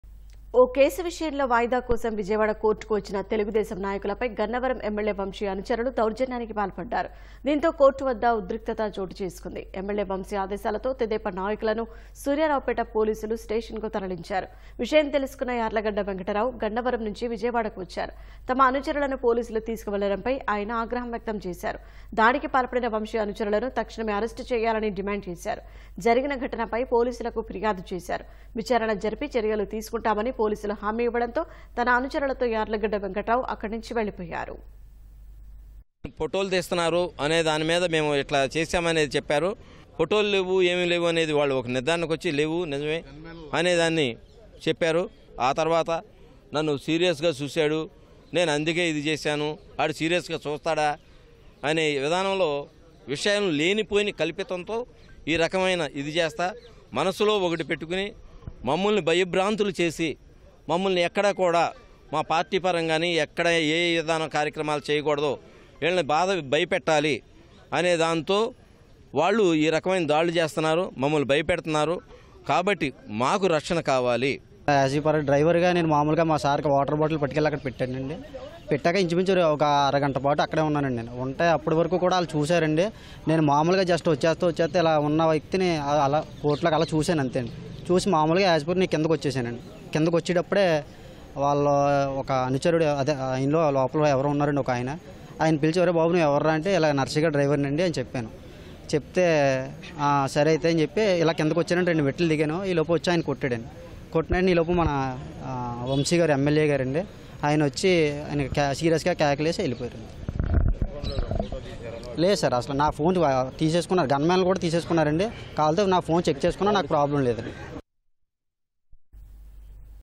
తెదేపాపై వంశీ అనుచరుల దాడి | Gannavaram MLA Vamshi Followers Attacked on TDP at Vijayawada Court
ఓ కేసు విషయంలో వాయిదా కోసం విజయవాడ కోర్టుకు వచ్చిన తెలుగుదేశం నాయకులపై గన్నవరం ఎమ్మెల్యే వంశీ అనుచరులు దౌర్జన్యానికి పాల్పడ్డారు. దీంతో కోర్టు వద్ద ఉద్రిక్తత చోటుచేసుకుంది. ఎమ్మెల్యే వంశీ ఆదేశాలతో తెదేపా నాయకులను సూర్యారావుపేట పోలీసులు స్టేషన్‌కు తరలించారు. విషయం తెలుసుకున్న యార్లగడ్డ వెంకటరావు గన్నవరం నుంచి విజయవాడకు వచ్చారు. తమ అనుచరులను పోలీసులు తీసుకువెళ్లడంపై ఆయన ఆగ్రహం వ్యక్తం చేశారు. దాడికి పాల్పడిన వంశీ అనుచరులను తక్షణమే అరెస్టు చేయాలని డిమాండ్  చేశారు. జరిగిన ఘటనపై పోలీసులకు ఫిర్యాదు చేశారు. విచారణ జరిపి చర్యలు తీసుకుంటామని పోలీసులు హామీ ఇవ్వడంతో తన అనుచరులతో యార్లగడ్డ వెంకట్రావు అక్కడ నుంచి వెళ్లిపోయారు.